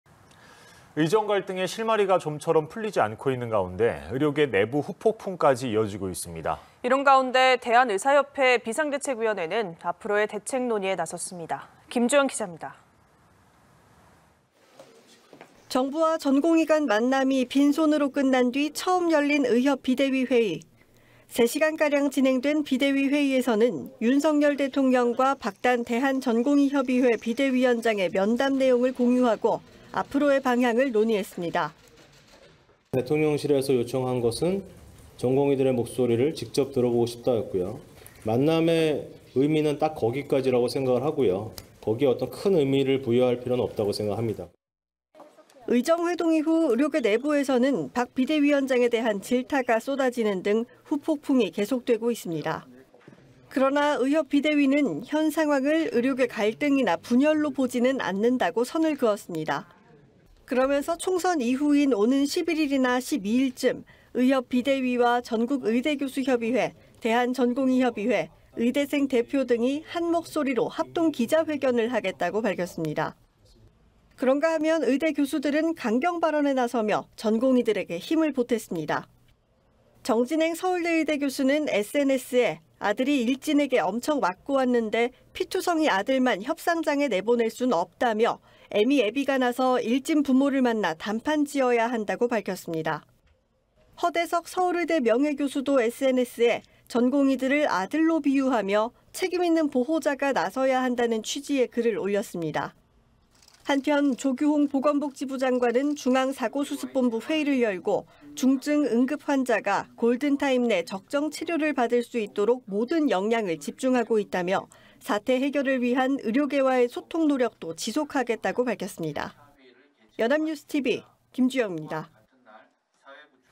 의정 갈등 장기화…의료계, 총선 이후 입장 낼듯 / 연합뉴스TV (YonhapnewsTV)
의정 갈등 장기화…의료계, 총선 이후 입장 낼듯

[앵커]

의정 갈등의 실마리가 좀처럼 풀리지 않고 있는 가운데, 의료계 내부 후폭풍까지 이어지고 있습니다.

이런 가운데 대한의사협회 비상대책위원회는 앞으로의 대책 논의에 나섰습니다.

김주영 기자입니다.

[기자]

정부와 전공의 간 만남이 빈 손으로 끝난 뒤 처음 열린 의협 비대위 회의.

세 시간 가량 진행된 비대위 회의에서는 윤석열 대통령과 박단 대한전공의협의회 비대위원장의 면담 내용을 공유하고 앞으로의 방향을 논의했습니다.

[김성근/의협 비대위 언론홍보위원장] "대통령실에서 요청한 건 전공의들의 목소리를 직접 들어보고 싶다였고요. 만남의 의미는 딱 거기까지라고 생각을 하고요. 거기에 큰 의미를 부여할 필요는 없다고 생각합니다."

의정 회동 이후 의료계 내부에서는 박 비대위원장에 대한 질타가 쏟아지는 등 후폭풍이 계속되고 있습니다.

그러나 의협 비대위는 현 상황을 의료계 갈등이나 분열로 보지는 않는다고 선을 그었습니다.

그러면서 총선 이후인 오는 11일이나 12일쯤 의협 비대위와 전국의대교수협의회, 대한전공의협의회, 의대생 대표 등이 한 목소리로 합동 기자회견을 하겠다고 밝혔습니다.

그런가하면 의대 교수들은 강경 발언에 나서며 전공의들에게 힘을 보탰습니다.

정진행 서울대 의대 교수는 SNS에 "아들이 일진에게 엄청 맞고 왔는데 피투성이 아들만 협상장에 내보낼 순 없다"며 "에미 애비가 나서 일진 부모를 만나 담판 지어야 한다"고 밝혔습니다.

허대석 서울의대 명예교수도 SNS에 전공의들을 '아들'로 비유하며 책임 있는 보호자가 나서야 한다는 취지의 글을 올렸습니다.

한편 조규홍 보건복지부 장관은 중앙사고수습본부 회의를 열고 "중증·응급환자가 골든타임 내 적정치료를 받을 수 있도록 모든 역량을 집중하고 있다"며 "사태 해결을 위한 의료계와의 소통 노력도 지속하겠다"고 밝혔습니다.

[영상취재기자 : 홍수호 이정우]